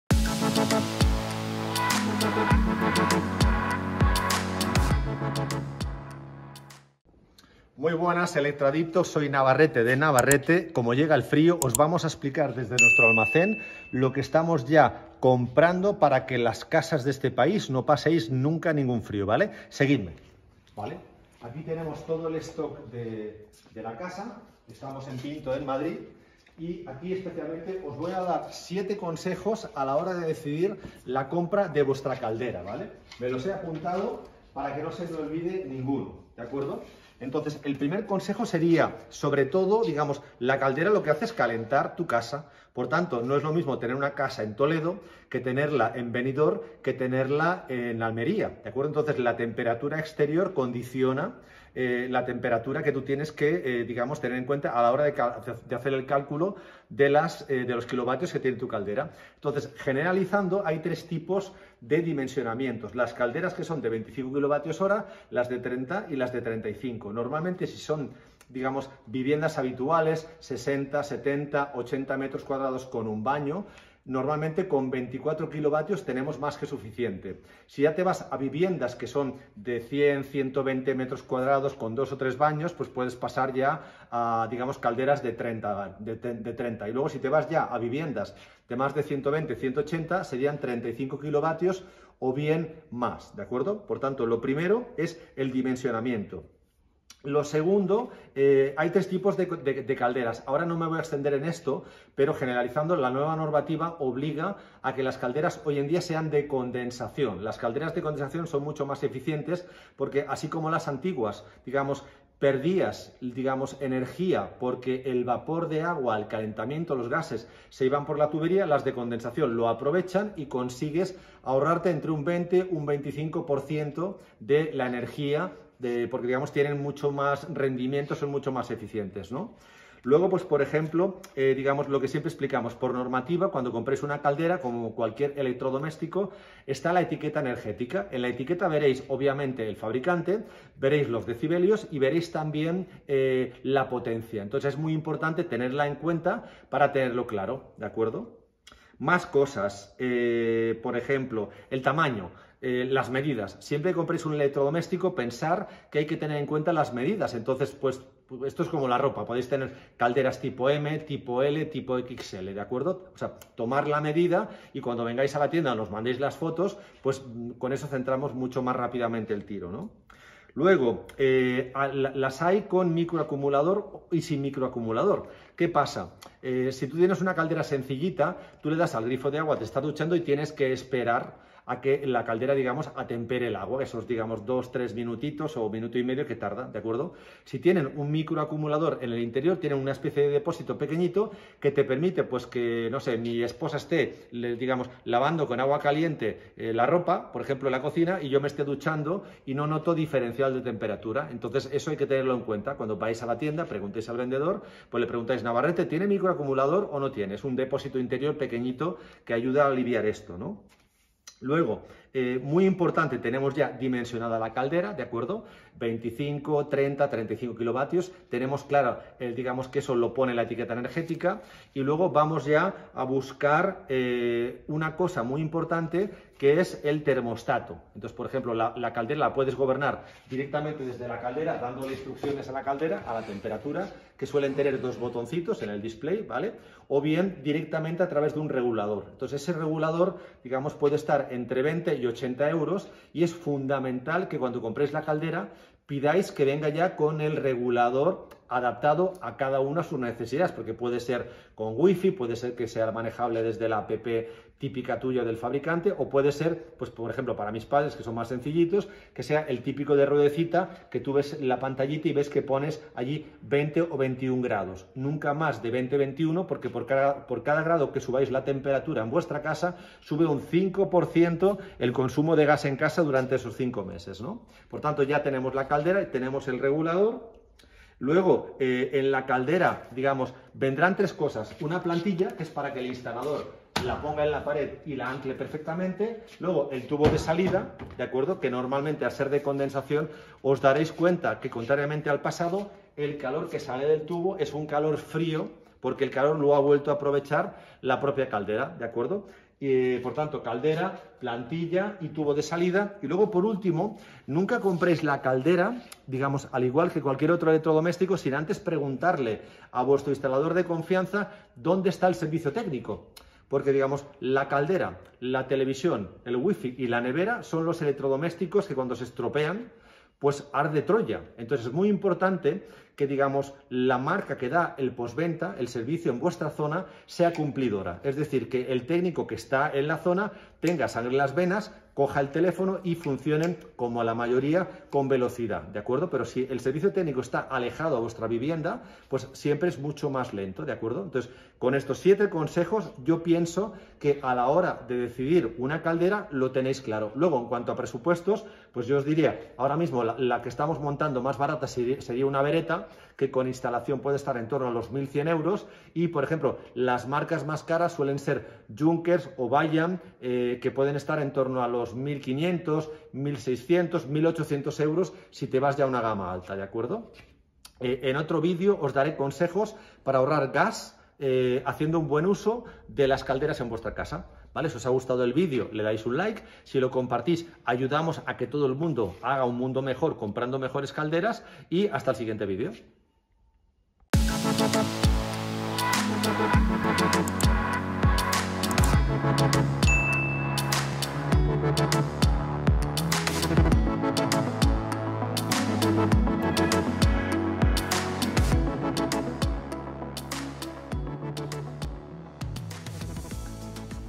▷Cómo comprar una caldera de gas y sacarle el mayor partido
⭐️ La caldera es uno de nuestros aparatos preferidos. Gracias a ella ganamos confort en nuestras casas. En este video te damos algunos consejos prácticos para que decidas con criterio y la uses sacándole el mayor partido.

¡Disfrútala!

🧭 Minutos 🧭

0:00 Introducción
0:38 Consejo 1
1:35 Consejo 2
2:11 Consejo 3
2:30 Consejo 4
2:50 Consejo 5
3:30 Consejo 6
4:37 Consejo 7